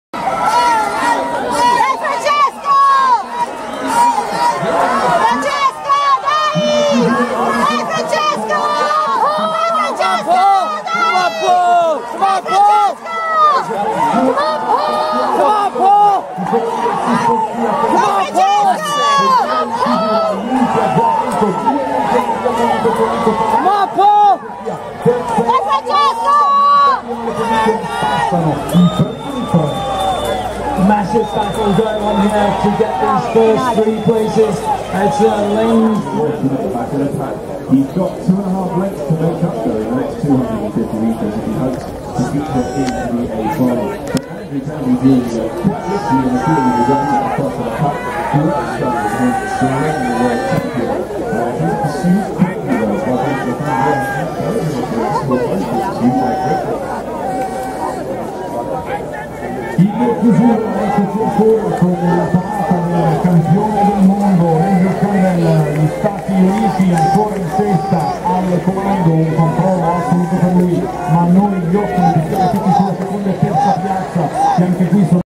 BLM1x Semifinals, WRU23Champs, Varese 2014
Close race in Semi 1 between Enes Kusku (TUR), Paul O'Donovan (IRL), and Francesco Pegoraro (ITA).
In Semi 2, a much more clear-cut race with Andew Campbell Jr, demolishing the field.